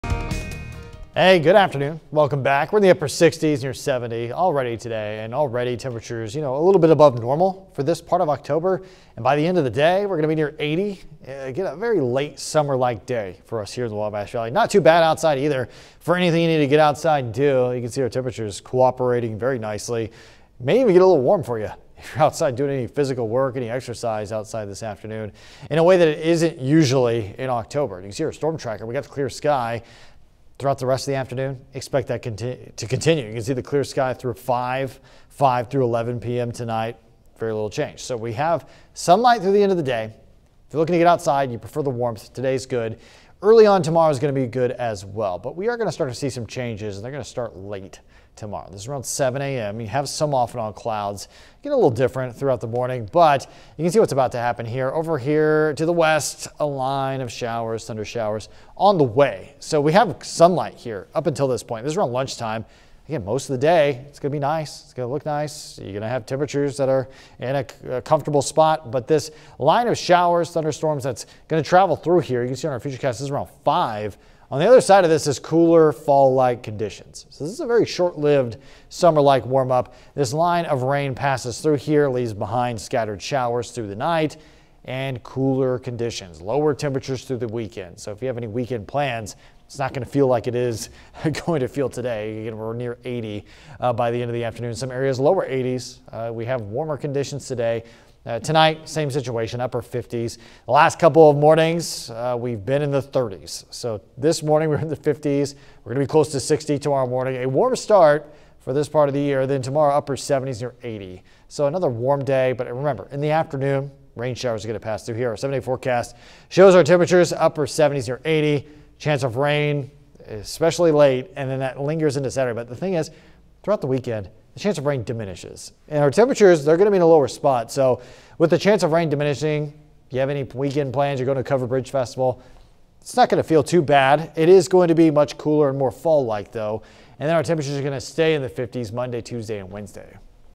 Nice and warm this afternoon, but watch for rain to end the work week!
Warmer weather this afternoon with plenty of sunshine!